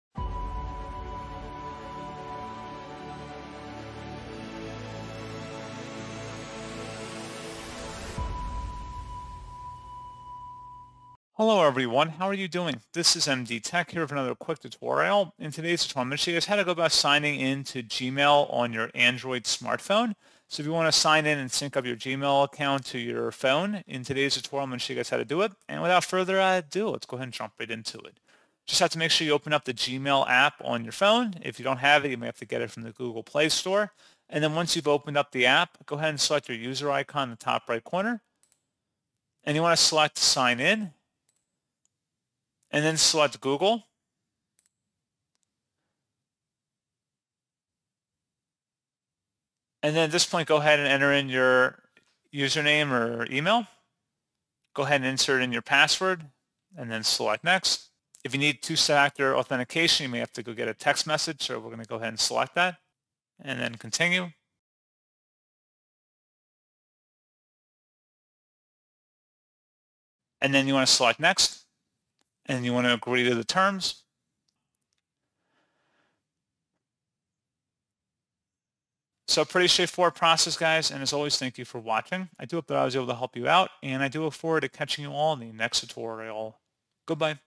How To Sign In To Gmail On Android [Guide]
By following the simple steps, you can easily add your Gmail account to the Gmail app or through your phone’s settings. Once signed in, you'll be able to manage your inbox, send and receive messages, and stay organized. This feature ensures seamless integration of your Google services, making it easier to stay on top of your emails while on the go.